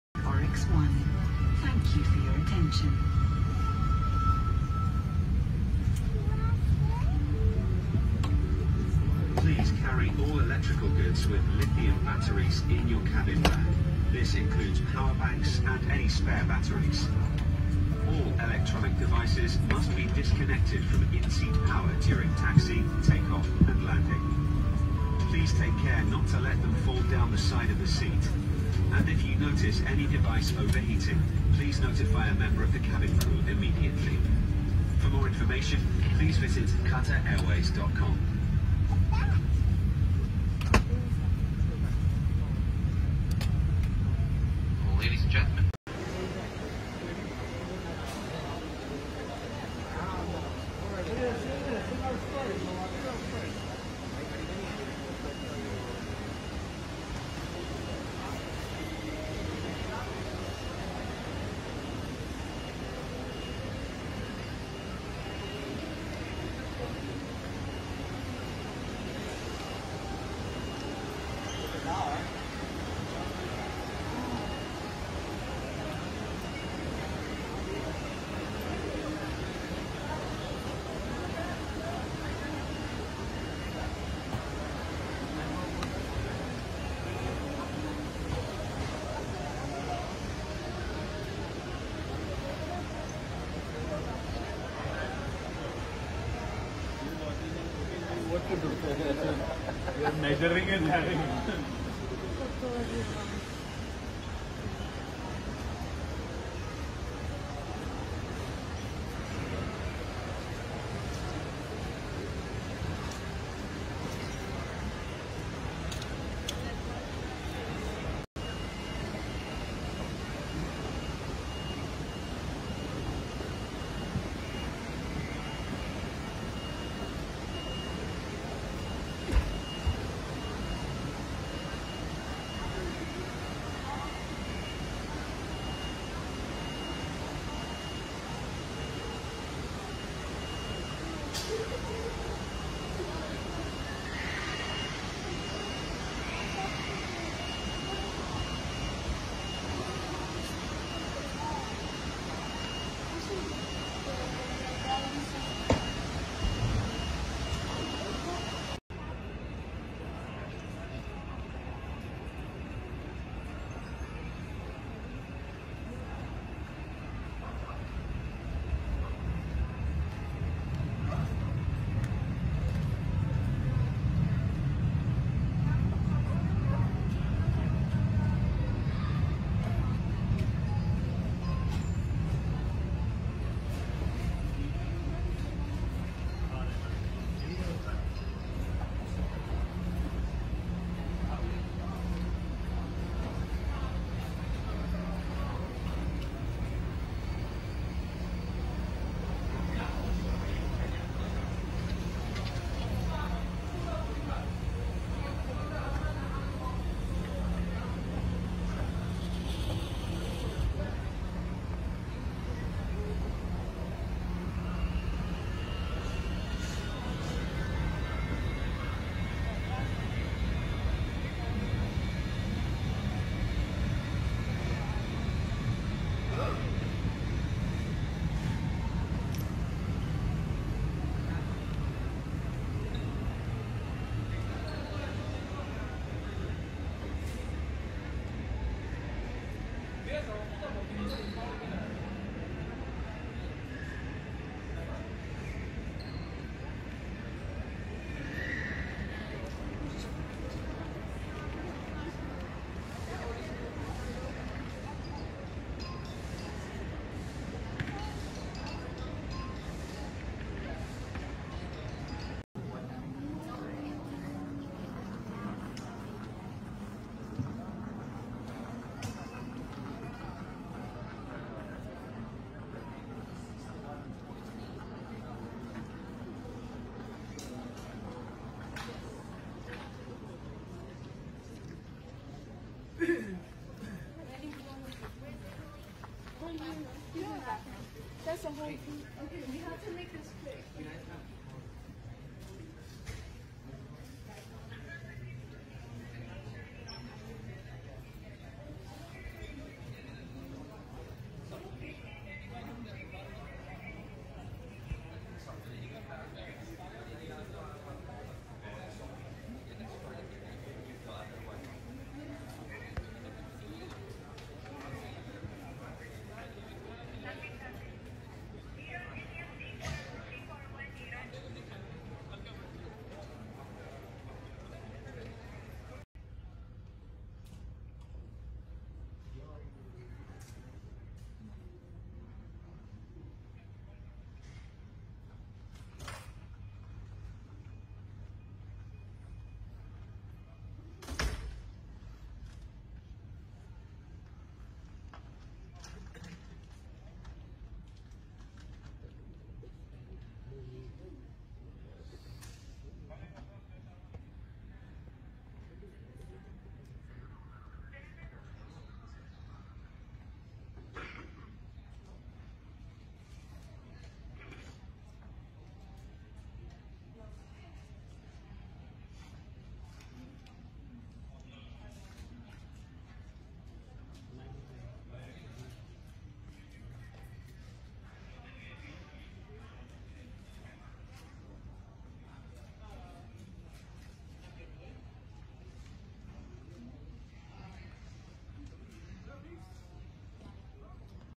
Doha (Qatar) international airport. / Ngắm nhìn một góc nhỏ sân bay Doha- Qatar ./Cuộc sống Mỹ
Ngắm nhìn một góc nhỏ sân bay của Doha- Qatar, sân bay của  Doha- Qatar rất là sạch đẹp, rộng lớn, hiện, bài bản, khi quá cảnh thì rất dễ tìm các ga kế tiếp.

Cao Nguyên USA, Cuộc sống Mỹ , Skateboarding, Môn ván trượt, chơi ván trượt, chơi patin, legos